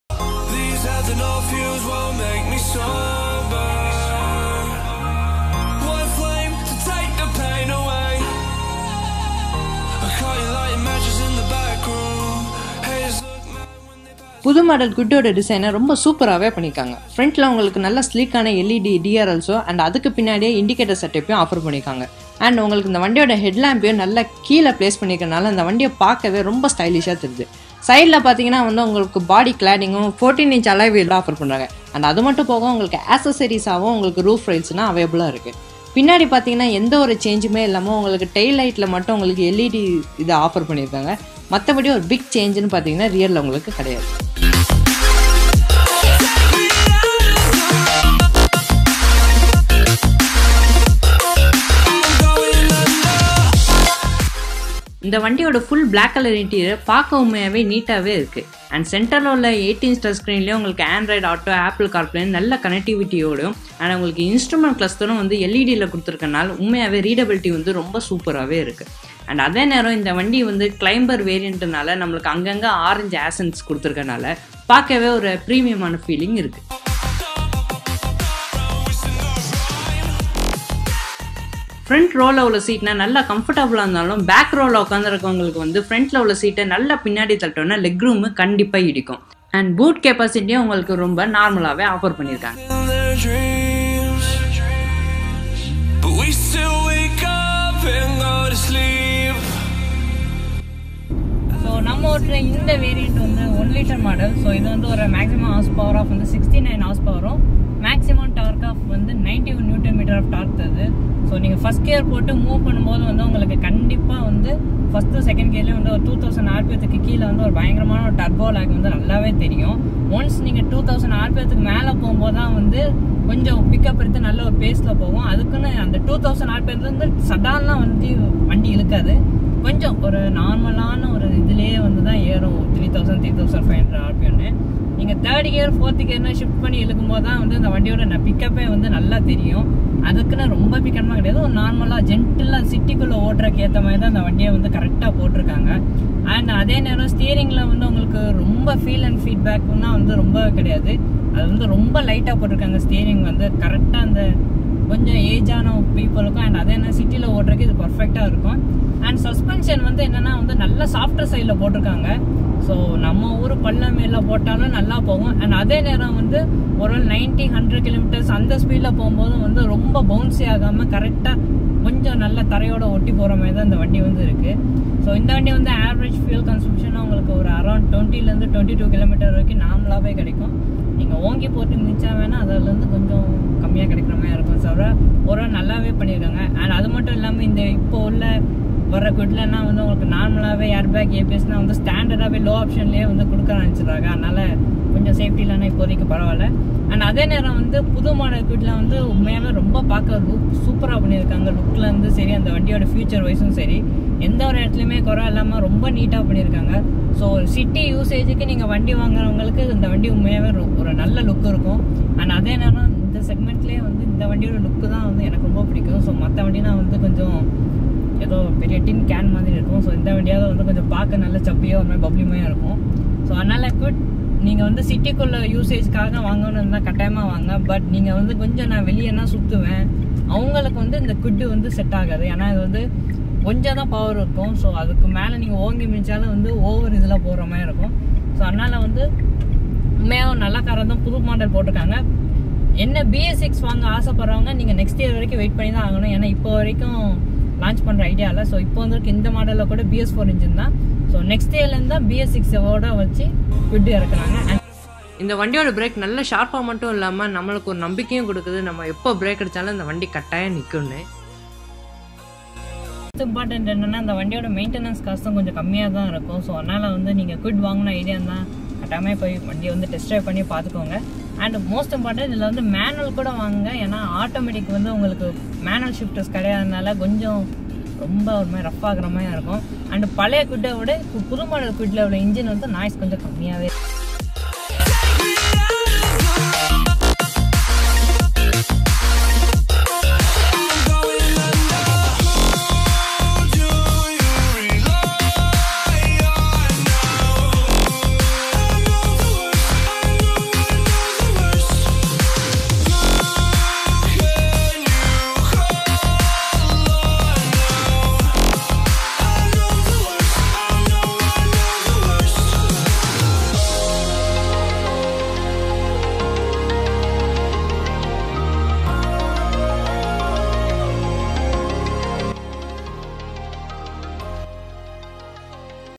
RENAULT KWID 2019 தமிழ் REVIEW / CARS 2 Wiki TAMIL
This video featuring the pros and cons of 2019 RENAULT KWID.

Also follow the Officials Cars 2  wiki on Insta